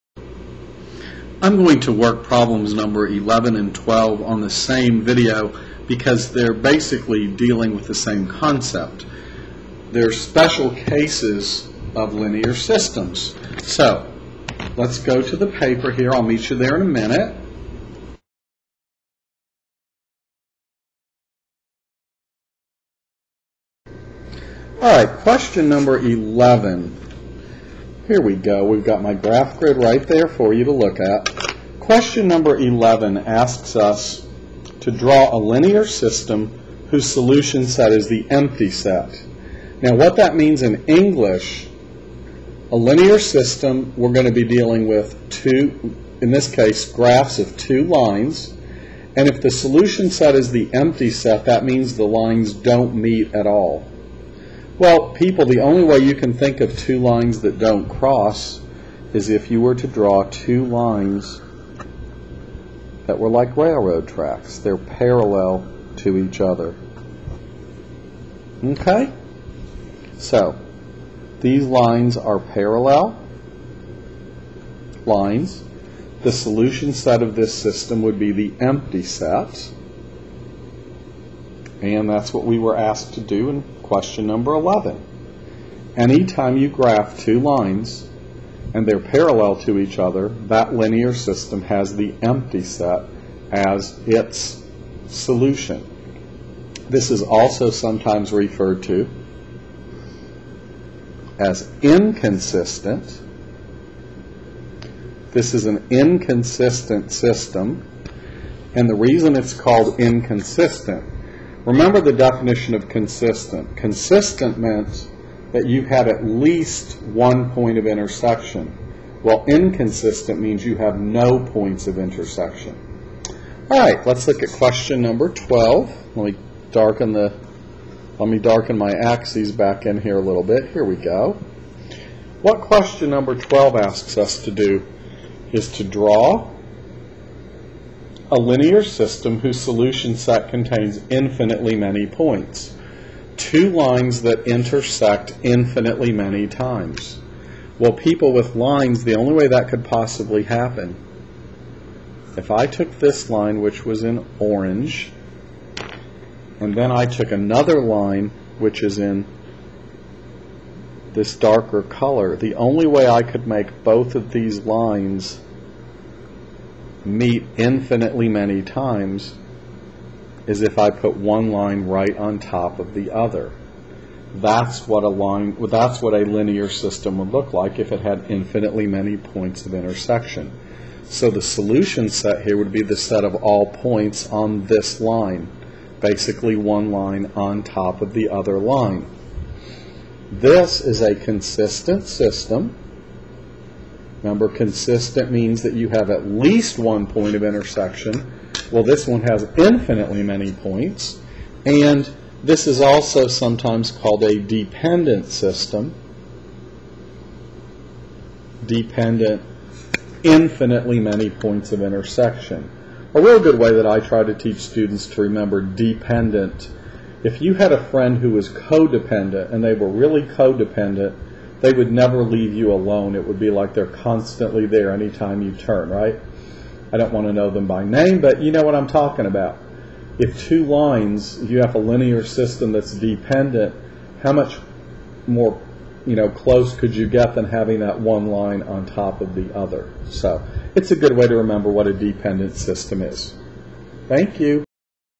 Test Two Review - Questions 11 and 12
linear systems with no solution
linear systems with infinitely many points of intersection